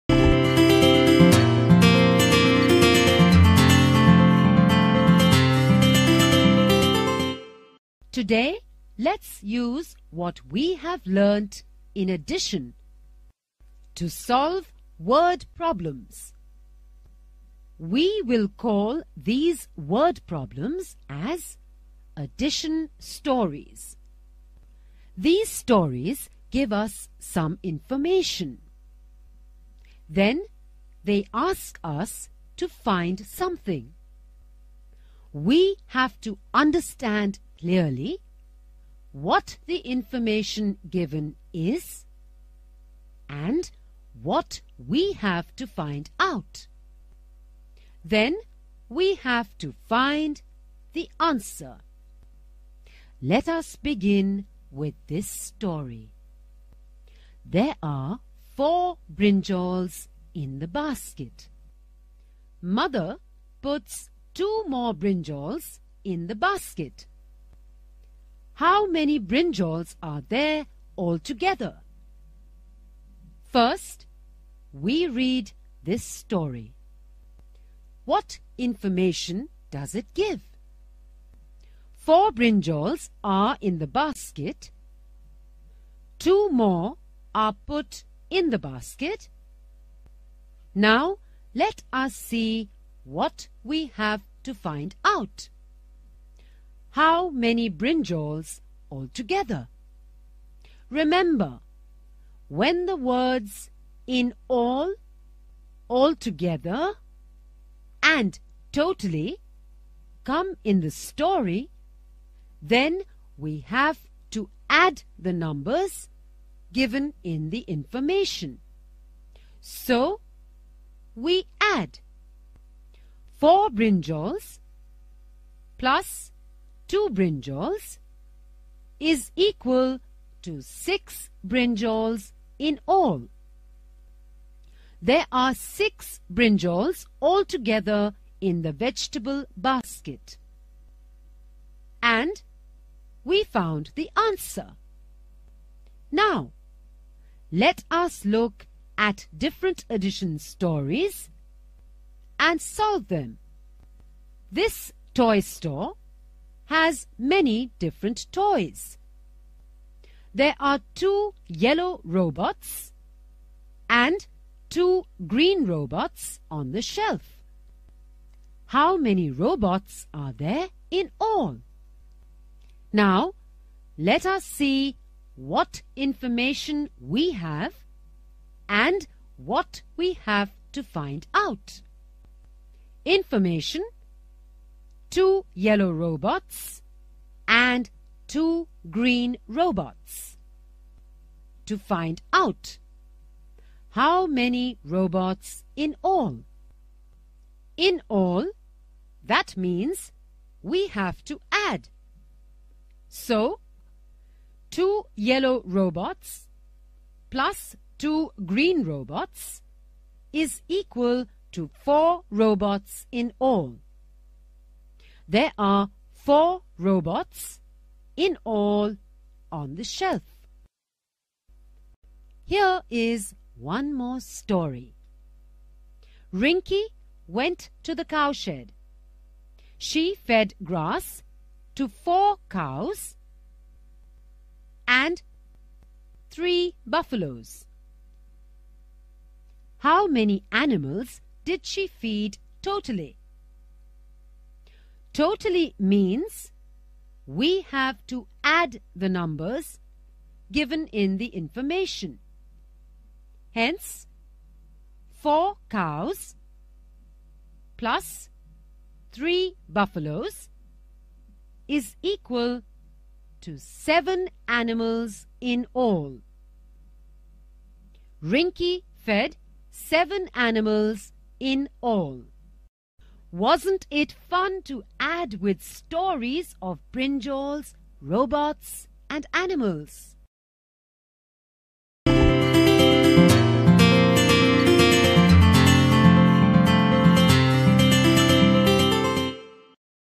Learn Addition
The City School, Shalimar Campus, Lahore
Class: Kindergarten
Subject: Mathematics
Topic: Learn Addition With The Help Of Addition Stories   Maths Concepts For Kids